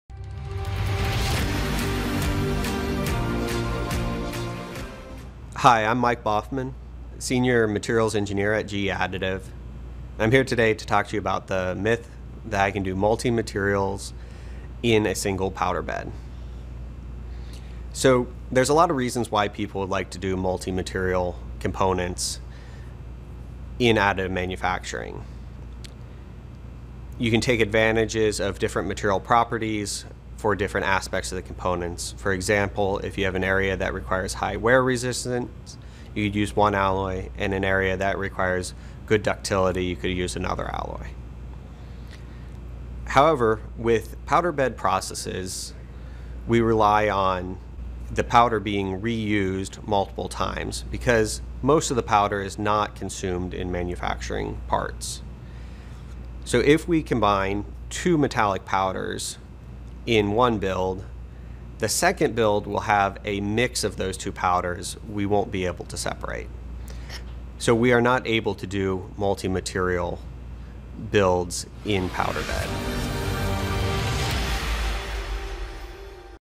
Materials Myth Conceptions: Use of Multiple Materials
The myth that it can do multi-materials in a single powder bed so there's a lot of reasons why people would like to do multi-material components in additive manufacturing you can take advantages of different material properties for different aspects of the components.